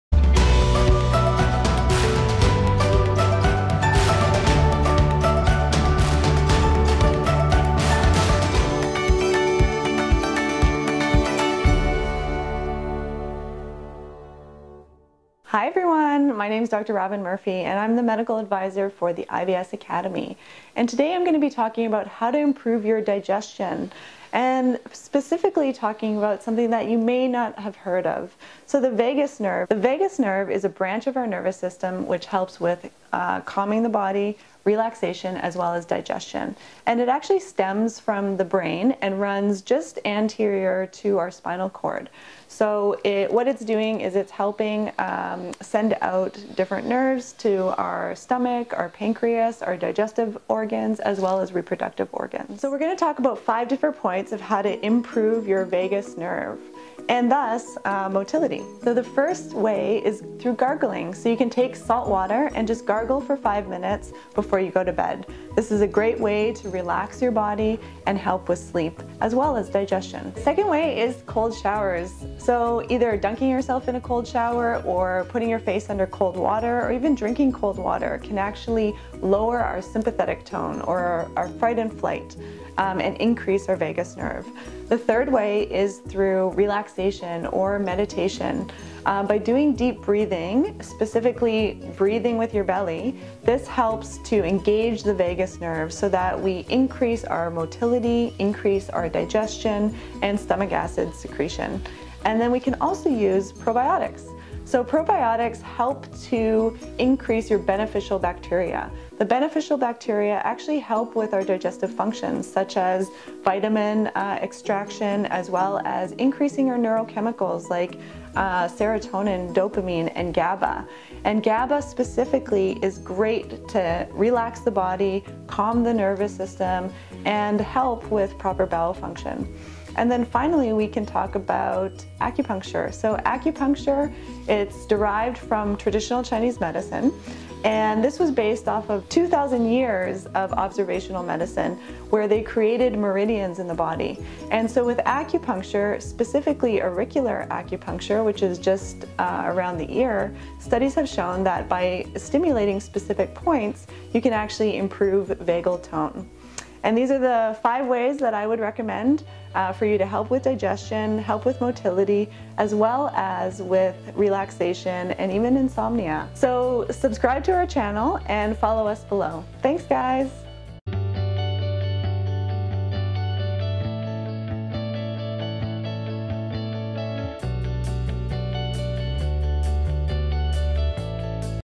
5 Ways to Improve Digestion (The KEY You May Not Know)
Today our Medical Advisor, Dr. Robyn Murphy, sheds some light on the one key to better digestion - supporting the vagus nerve! Learn how to improve digestion with these 5 unique ways.

The IBS Academy